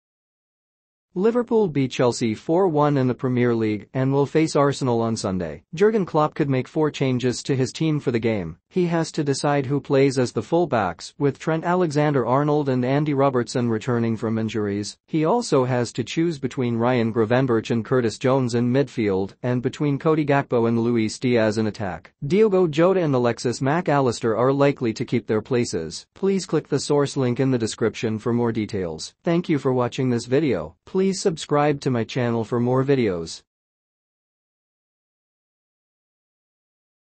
Liverpool could make four changes against Arsenal as Jurgen Klopp handed new headache
Liverpool beat Chelsea 4-1 in the Premier League and will face Arsenal on Sunday. Jurgen Klopp could make four changes to his team for the game. He has to decide who plays as the full-backs, with Trent Alexander-Arnold and Andy Robertson returning from injuries. He also has to choose between Ryan Gravenberch and Curtis Jones in midfield, and between Cody Gakpo and Luis Diaz in attack. Diogo Jota and Alexis Mac Allister are likely to keep their places.